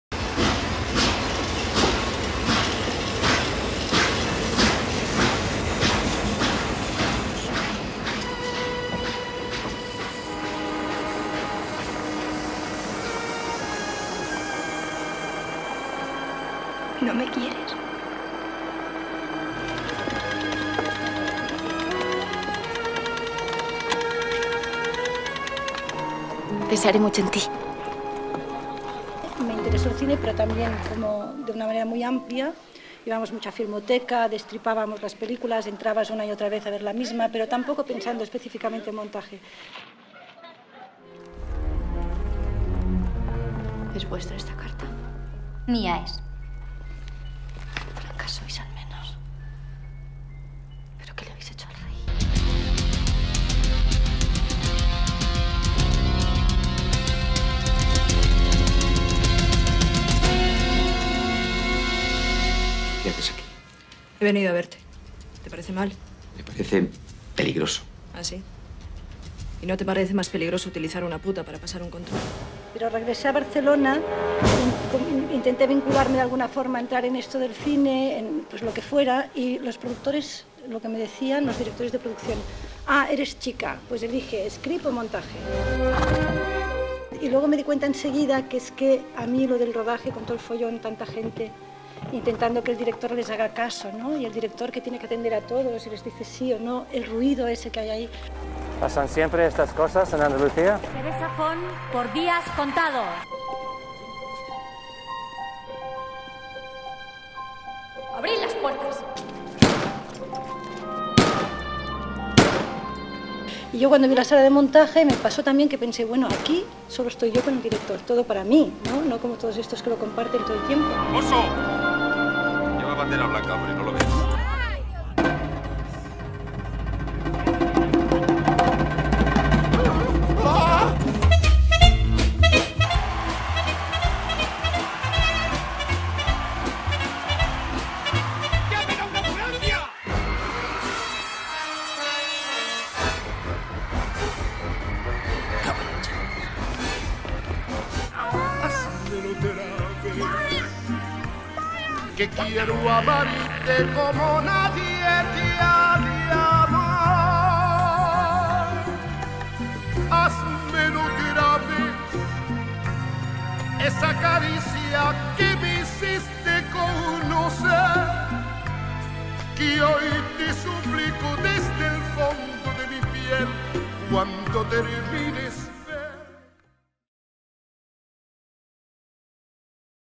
Ricardo Franco Spanish Films Academy Award to Teresa Font HD
Video for the tribute show to Teresa Font (film editor) in Cervantes Theatre (19th Málaga Spanish Film Festival)